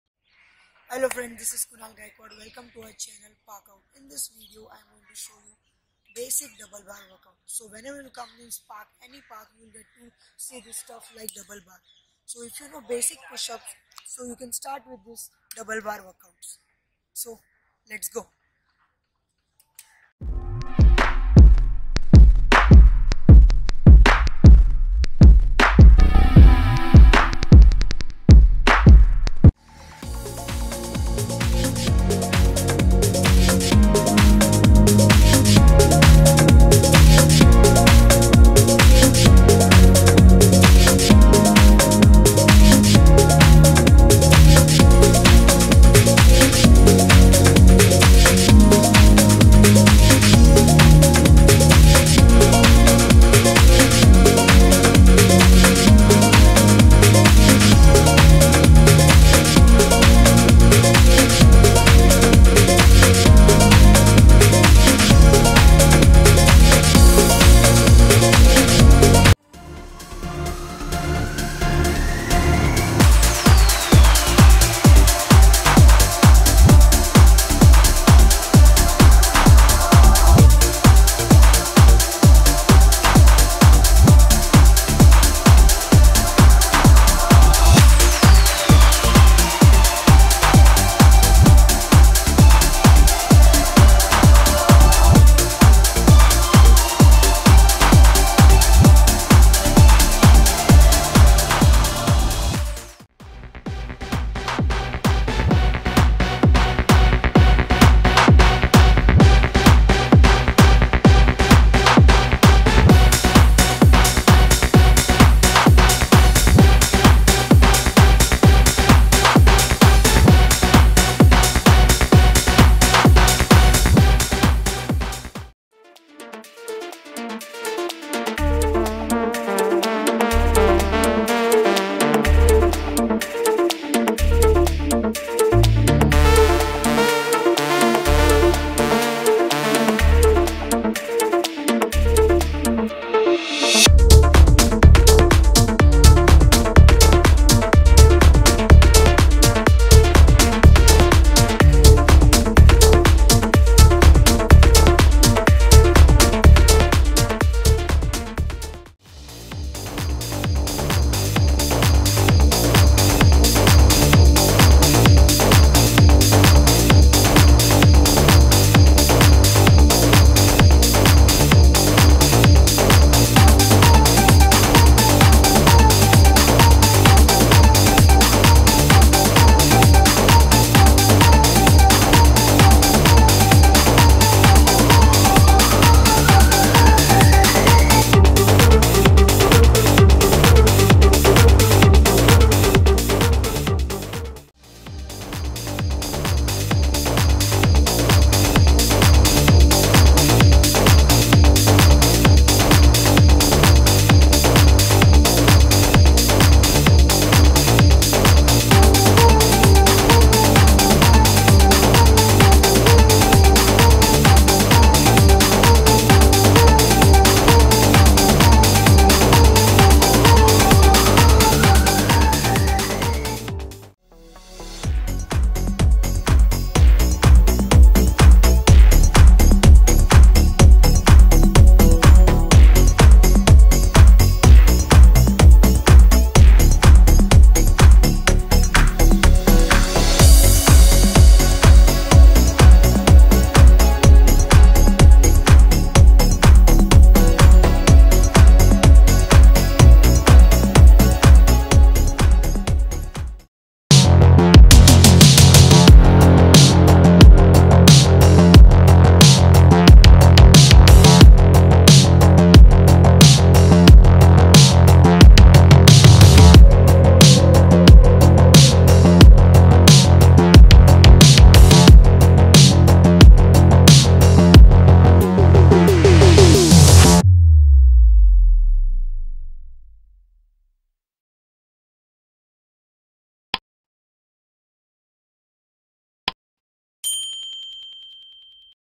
Basic Double-Bar Workout | Parallel Bar Workout | Kunal Gaikwad
Hello Everyone!!! Welcome Once Again! Here is my new video based on Exercises which you can do on Double-Bar or you can also say Parallel-Bars. These Set of Exercises help you to develop most of your Upper body muscle making them strong and also keeping them in good shape. Hope you will like this Video.
SUPPORT❤️